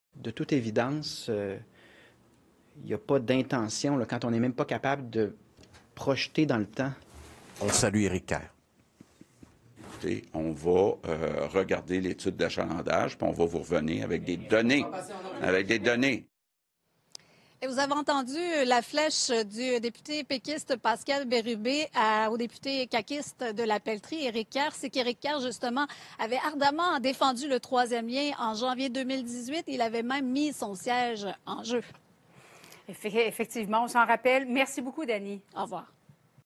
Rien dans le budget pour le 3e lien : est-ce qu'Éric Caire va démissionner?
En janvier 2018, le grand Éric Caire avait mis son siège en jeu si le 3e lien n'allait pas de l'avant.